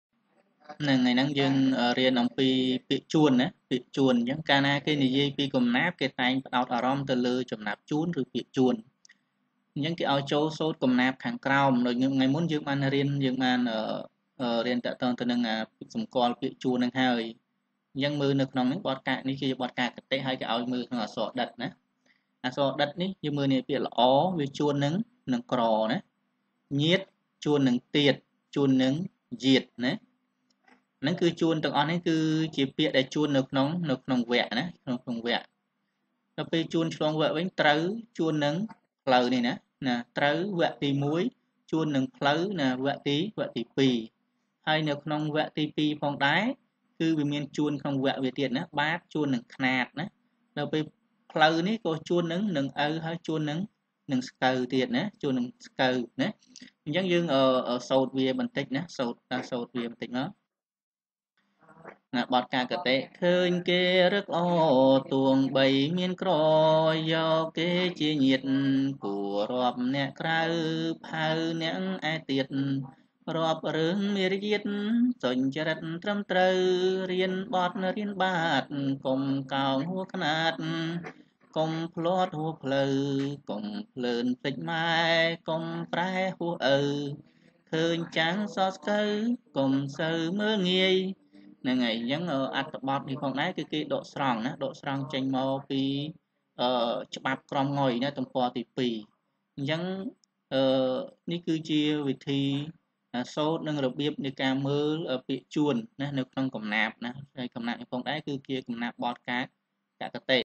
កំណាព្យ ច្បាប់ក្រមង៉ុយ បង្រៀនដោយលោកគ្រូ ញ៉ឹម ញីម (Nhem Nhim)
មេរៀនទាំងឡាយនៅក្នុងសៀវភៅភាសាខ្មែរនានា!